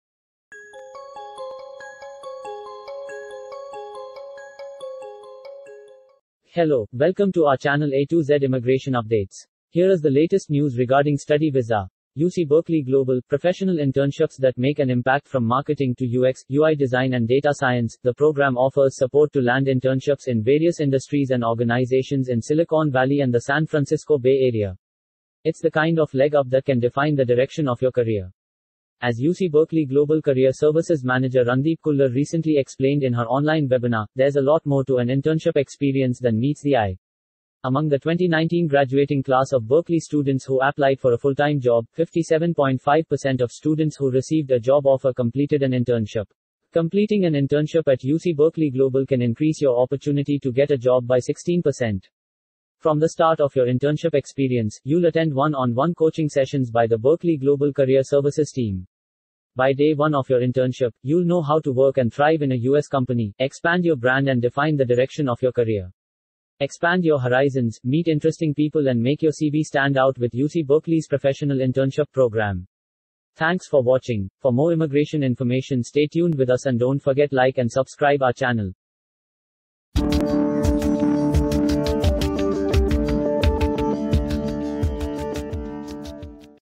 UC Berkeley Global Professional internships that make an impact
Video Description: From marketing to UX/UI design and data science, the programme offers support to land internships in various industries and organisations in Silicon Valley and the San Francisco Bay Area. It’s the kind of leg-up that can define the direction of your career. As UC Berkeley Global Career Services Manager Randeep Kullar recently explained in her online webinar, there’s a lot more to an internship experience than meets the eye. Among the 2019 graduating class of Berkeley students who applied for a full-time job, 57.5% of students who received a job offer completed an internship. Completing an internship at UC Berkeley Global can increase your opportunity to get a job by 16%. From the start of your internship experience, you’ll attend one-on-one coaching sessions by the Berkeley Global Career Services team. By day one of your internship, you’ll know how to work and thrive in a US company, expand your brand and define the direction of your career. Expand your horizons, meet interesting people and make your CV stand out with UC Berkeley’s Professional Internship Programme.